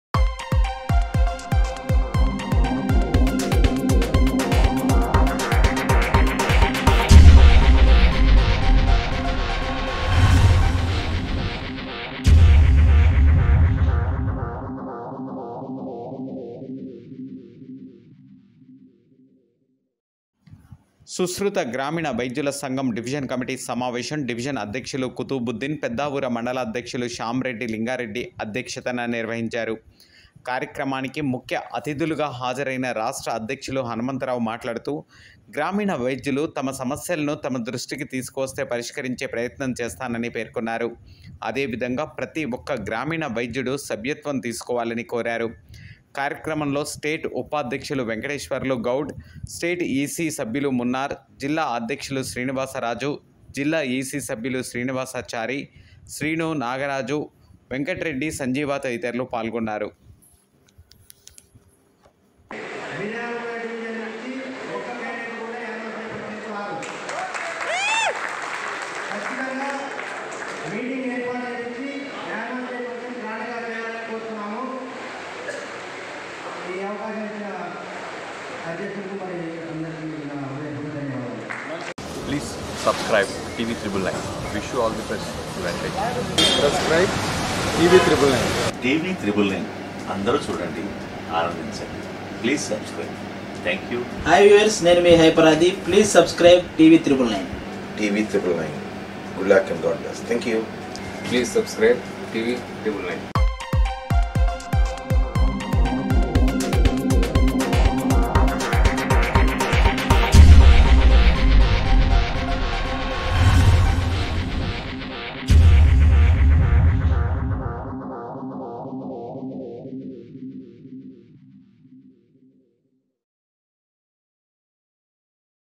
సుశ్రుత గ్రామీణ వైద్యుల సంఘం మిర్యాలగూడలో డివిజన్ మీటింగ్ 30.03.22 న బుధవారం నిర్వహించడం జరిగింది
సుశ్రుత గ్రామీణ వైద్యుల సంఘం మిర్యాలగూడలో డివిజన్ మీటింగ్ 30.03.22 న బుధవారం నిర్వహించడం జరిగింది డివిజన్ అధ్యక్షులు కుతుబద్దీన్ గారు మరియు పెద్దవూర మండల అధ్యక్షులు శ్యామ్ రెడ్డి లింగారెడ్డి గారి అధ్యక్షత న  ఈరోజు నిర్వహించడం జరిగింది  ఈ కార్యక్రమంలో ముఖ్య అతిథులుగా రాష్ట్ర అధ్యక్షులు హన్మంతరావు గారు  హాజరయ్యారు  రాష్ట్ర అధ్యక్షులు గారు మాట్లాడుతూ సంఘ సబ్యులకు ఉన్నటువంటి సమస్యలు ఏవైనా ఉంటే తన దృష్టికి తీసుకురావాలని అలాగే రాష్ట్రంలో ని ప్రతి సభ్యుడు కూడా ఖచ్చితంగా సభ్యత్వం తీసుకోవాలి అని తెలియజేసారు ఈకార్యక్రమంలో స్టేట్ వైస్ ప్రెసిడెంట్  వెంకటేశ్వర్లు  గౌడ్,స్టేట్ E. C మెంబర్ మున్నార్, డిస్ట్రిక్ట్ ప్రెసిడెంట్ శ్రీనివాస్ రాజు,డిస్ట్రిక్ట్ EC మెంబర్ శ్రీనివాసచారీ,  రావుల శ్రీను,నాగరాజు,వెంకటరెడ్డి,సంజీవ తదితరులు పాల్గొన్నారు